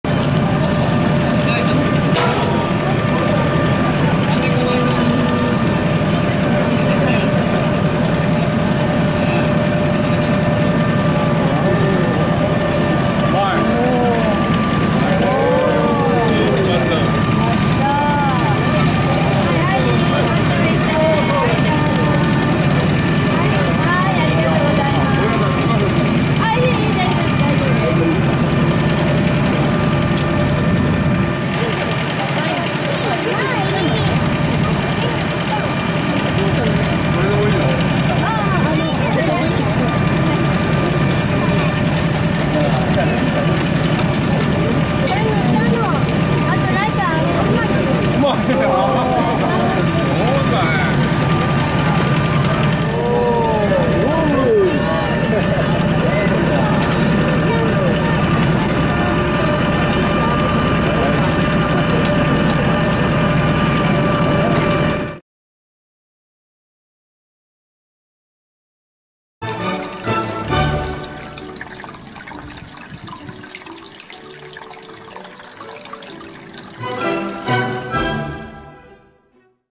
子どもが喜ぶ！！本物のパワーショベルで人形・スーパーボールすくい！！！
↓↓↓大日本発動機帝国 発動機士タナボタクボタの旅 日本発動機士会